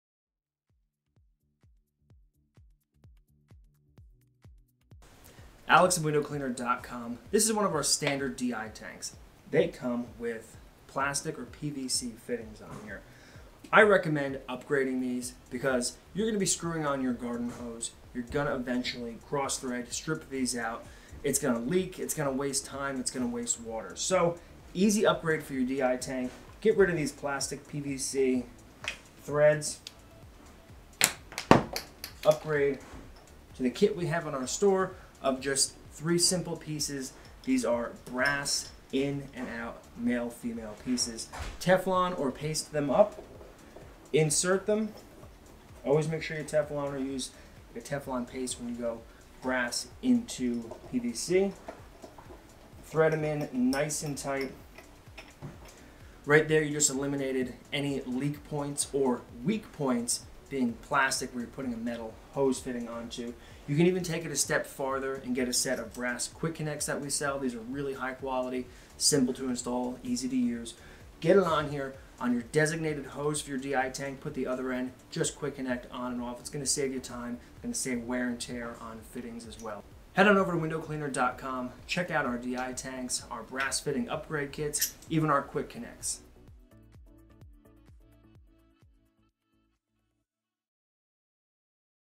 Upgrading Fittings on Your Di Tank
Our standard DI tanks will come with plastic or PVC fittings. We recommend upgrading these to the brass fittings. It's easy to install these new fittings and Alex will show you how in this video!